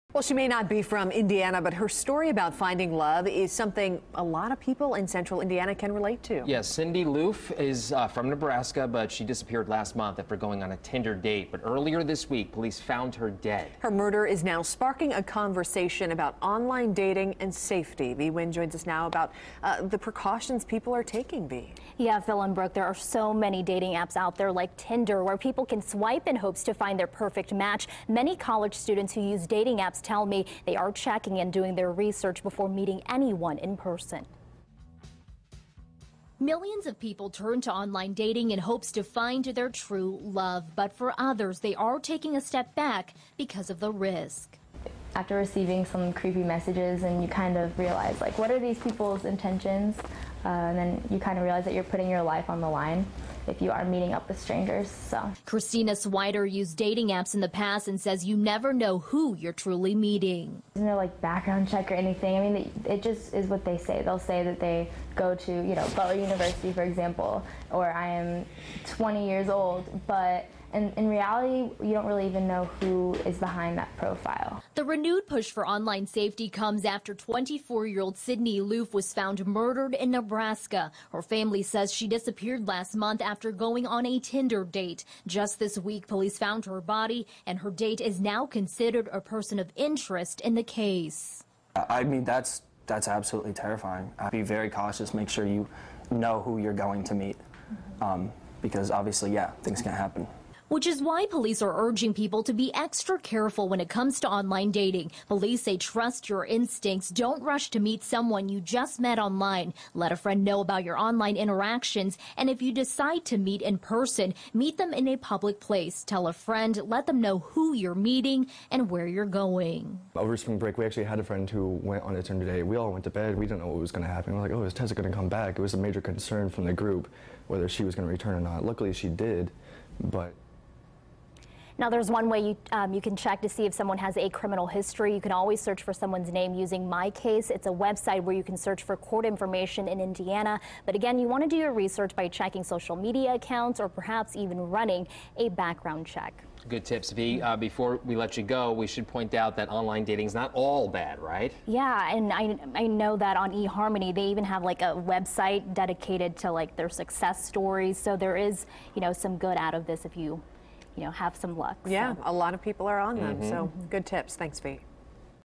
Online dating safety
24-Hour News 8's Vi Nguyen reports.